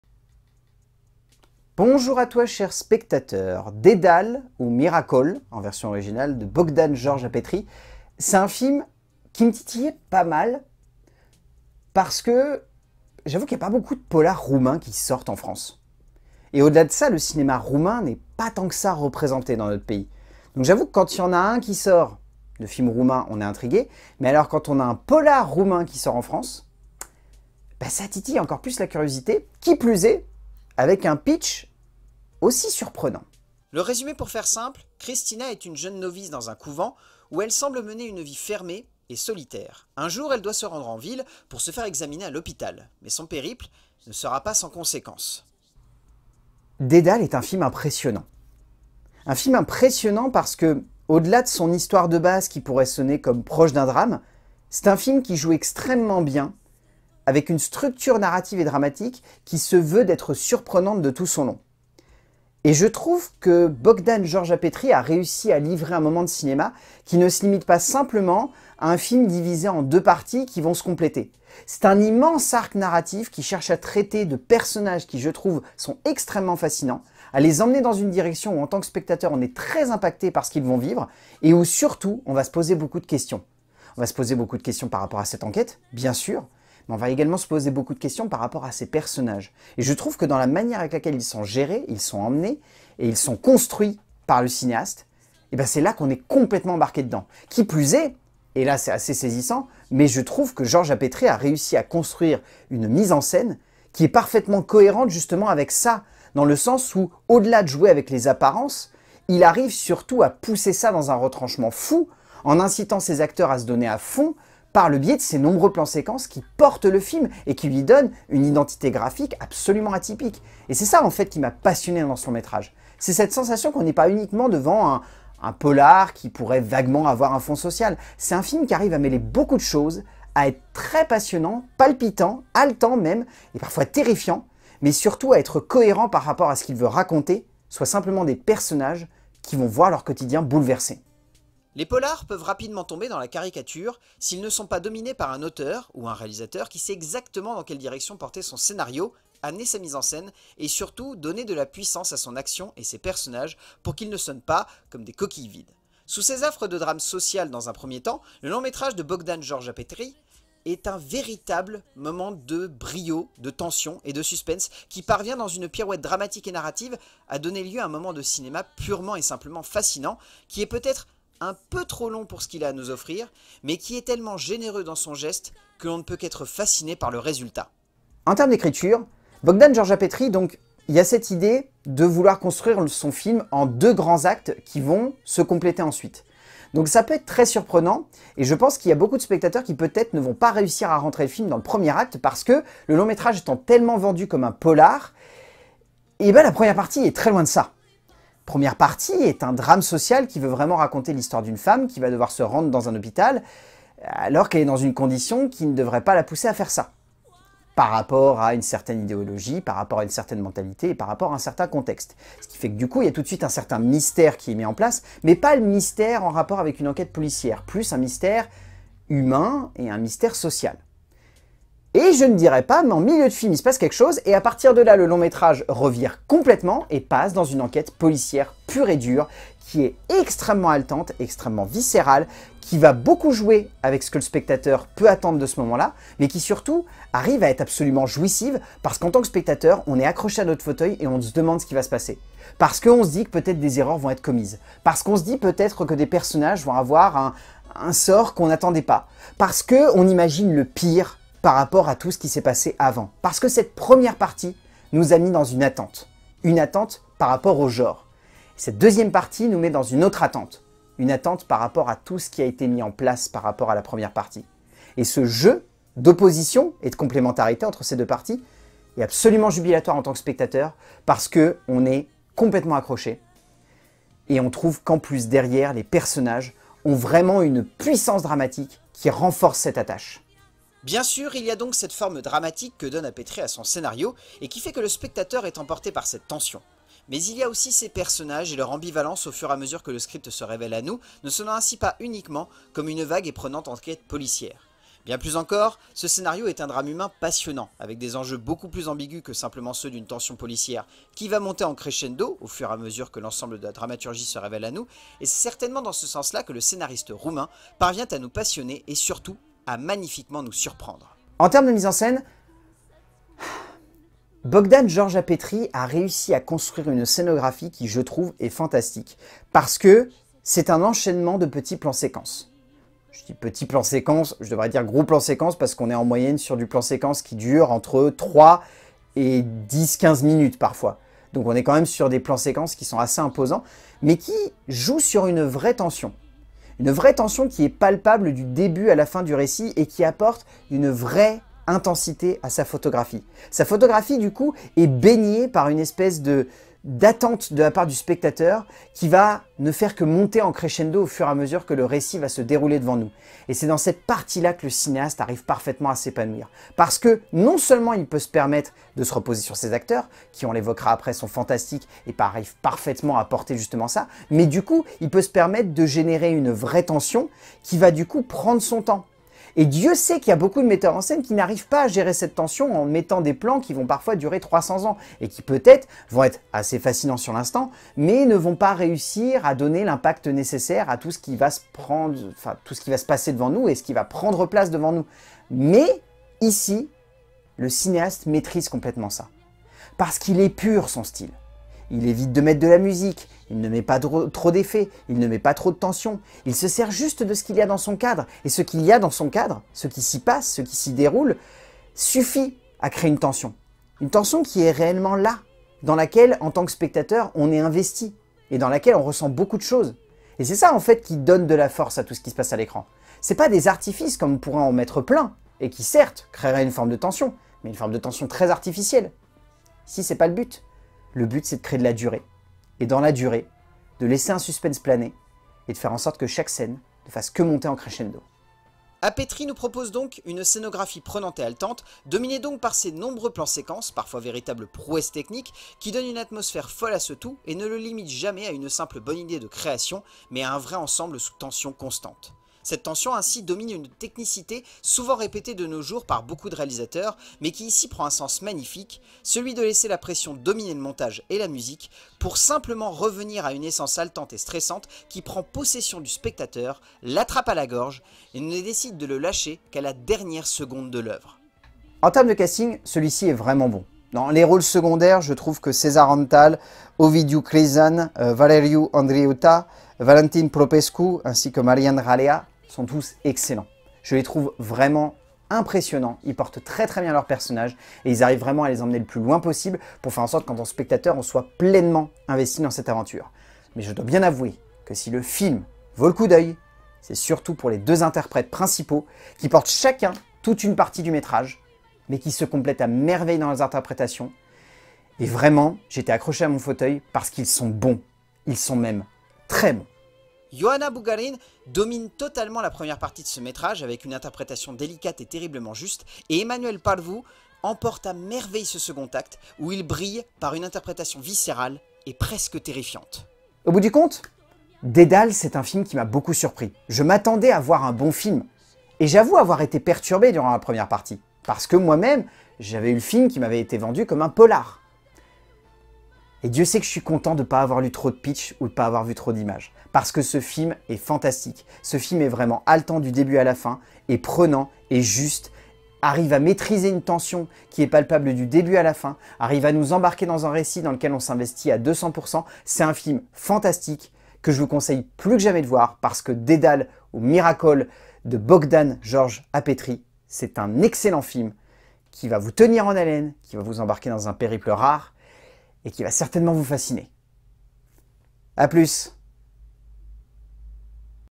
LCDC - Dédales
Chronique critique sur la sortie d'un thriller venant d'un lointain pays, dont on a pas forcément l'habitude de voir tant de long métrage que cela, mais franchement, si c'est de ce calibre, vivement que l'on en est encore, "Dédales"